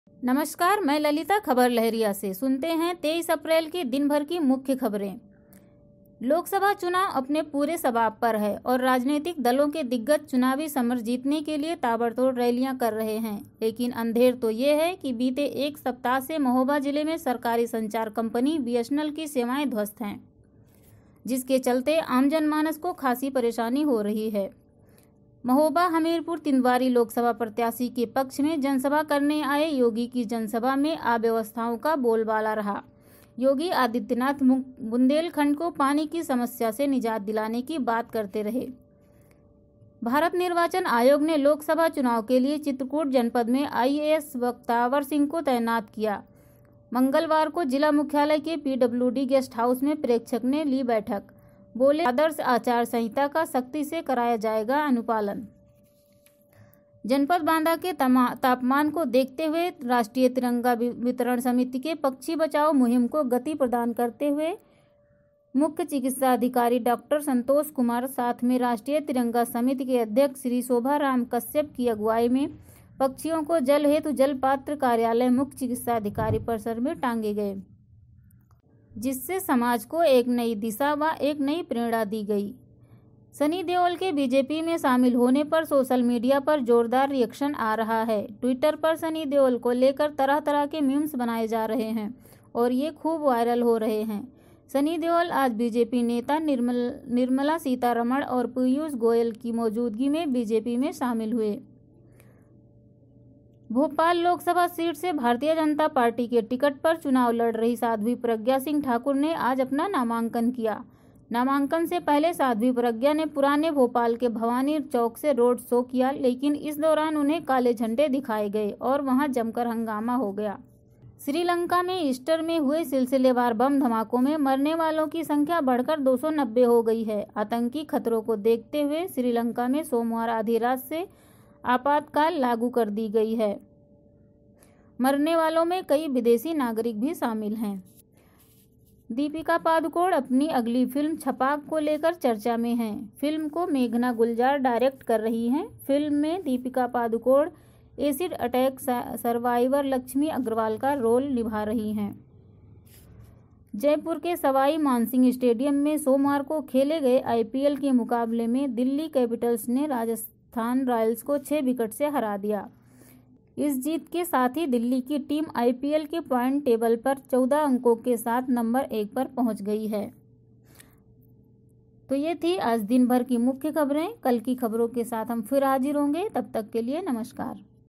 दिनभर की बड़ी खबरें सिर्फ खबर लहरिया पर | Fatafat Khabrein | KhabarLahariya
लोकसभा चुनाव अपने पूरे शबाब पर है और राजनैतिक दलों के दिग्गज चुनावी समर जितने के लिए ताबड़तोड़ रैलियां कर रहे हैं , लेकिन अंधेर तो यह है कि बीते एक सप्ताह से महोबा जिले में सरकारी संचार कंपनी बीएसएनएल की सेवाएं ध्वस्त हैं जिसके चलते आमजनमानस को खासी परेशानी हो रही है

महोबा हमीरपुर तिंदवारी लोकसभा प्रत्याशी के पक्ष में जनसभा करने आये योगी की जनसभा में अव्यवस्थाओं का रहा बोलबाला ।योगी आदित्यनाथ बुन्देलखण्ड को पानी की गम्भीर समस्या से निजात देने की करते रहे हैं बात

भारत निर्वाचन आयोग ने लोकसभा चुनाव के लिए चित्रकूट जनपद में I.A.S बख्तावर सिंह को तैनात किया प्रेक्षक

मंगलवार को जिला मुख्यालय के पीडब्ल्यूडी गेस्ट हाउस मे प्रेक्षक ने ली बैठक

बोले आदर्श आचार संहिता का शक्ति से कराया जाएगा अनुपालन

जनपद बाँदा के तापमान को देखते हुए राष्ट्रीय तिरंगा वितरण समिति के पक्षी बचाओ मुहिम को गति प्रदान करते हुए मुख्य चिकित्सा अधिकारी डॉ संतोष कुमार साथ मे राष्ट्रीय तिरंगा समिति के अध्यक्ष श्री शोभाराम कश्यप की अगुवाई में पछियों को जल हेतु जल पात्र कार्यालय मुख्य चिकित्सा अधिकारी परिसर में टांगे गए।जिससे समाज को एक नई दिशा,वा एक नई प्रेरणा दी गई

सनी देओल के बीजेपी में शामिल होने पर सोशल मीडिया पर जोरदार रिएक्शन आ रहा है. ट्विटर पर सनी देओल को लेकर तरह-तरह के Memes बनाए जा रहे हैं और ये खूब वायरल हो रहे हैं. सनी देओल आज बीजेपी नेता निर्मला सीतारमण और पीयूष गोयल की मौजूदगी में बीजेपी मे शामिल हुए

भोपाल लोकसभा सीट से भारतीय जनता पार्टी के टिकट पर चुनाव लड़ रहीं साध्वी प्रज्ञा सिंह ठाकुर ने आज अपना नामांकन किया. नामांकन से पहले साध्वी प्रज्ञा ने पुराने भोपाल के भवानी चौक से रोड शो किया, लेकिन इस दौरान उन्हें काले झंडे दिखाए गए और यहां जमकर हंगामा हो गया.

श्रीलंका में ईस्टर में हुए सिलसिलेवार बम धमाकों में मरने वालों की संख्या बढ़कर 290 हो गई है। आतंकी खतरों को देखते हुए श्रीलंका में सोमवार आधी रात से आपातकाल लागू कर दी गई। मरने वालों में कई विदेशी नागरिक शामिल हैं। इन धमाको में डेनमार्क के सबसे अमीर व्यक्ति एन्डर्स होल्च पॉवल्सन के 3 बच्चों की भी मौत हो गई है।

बीजेपी ने उत्तर पश्चिमी दिल्ली लोकसभा सीट से पंजाबी सिंगर हंसराज हंस को अपना उम्मीदवार बनाया है। इस सुरक्षित सीट से उन्हें मौजूदा सांसद उदितराज की जगह पर पार्टी का टिकट दिया गया है। हंसराज हंस बीते कई सालों से बीजेपी से जुड़े हुए हैं

दीपिका पादुकोण अपनी अगली फिल्म 'छपाक' को लेकर चर्चा में हैं । फिल्म को मेघना गुलजार डायरेक्ट कर रही हैं । फिल्म में दीपिका पादुकोण एसिड अटैक सर्वाइवर लक्ष्मी अग्रवाल का रोल निभा रही हैं

जयपुर के सवाई मानसिंह स्टेडियम में सोमवार को खेले गए आईपीएल के मुकाबले में दिल्ली कैपिटल्स ने राजस्थान रॉयल्स को 6 विकेट से हरा दिया. इस जीत के साथ ही दिल्ली की टीम आईपीएल के प्वाइंट टेबल में 14 अंको के साथ नंबर एक पर पहुंच गई है